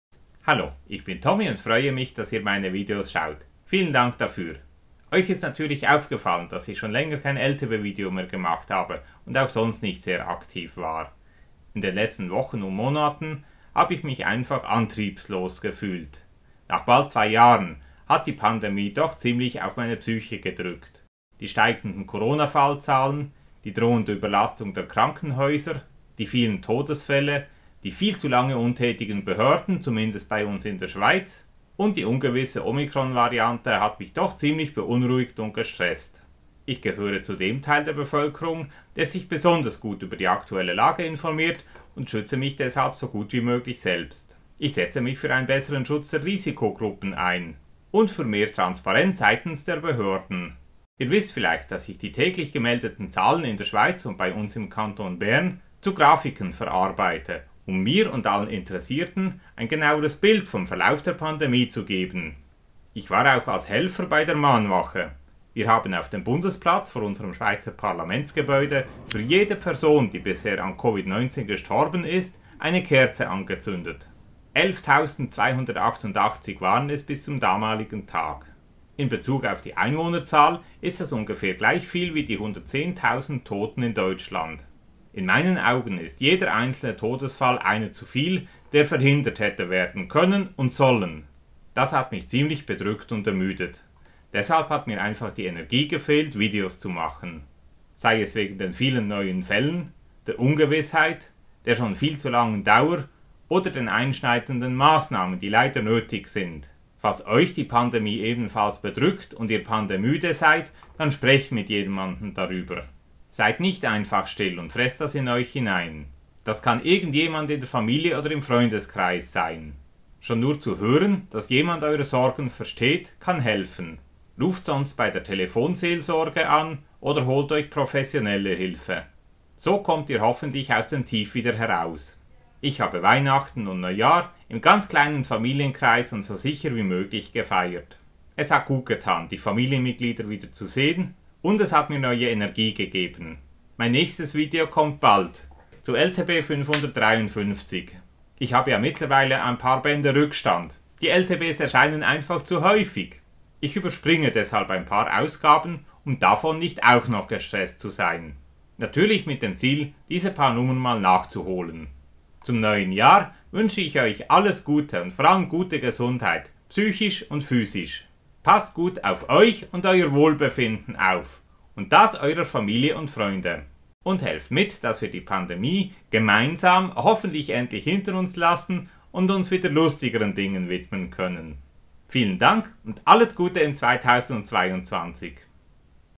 Pandemüde / Alles Gute im 2022!
Die Pandemie hat mich in letzter Zeit ziemlich bedrückt und ermüdet. Da hat mir einfach die Energie gefehlt, Videos zu machen. Jetzt fühle ich mich wieder besser. Mein nächstes Video kommt bald, zu LTB 553. Ich überspringe ein paar Lustige Taschenbücher, um davon nicht auch noch gestresst zu sein. Zum neuen Jahr wünsche ich Euch gute Gesundheit, psychisch und physisch. Passt gut auf Euch auf. Alles Gute im 2022, und danke, dass Ihr meine Videos schaut.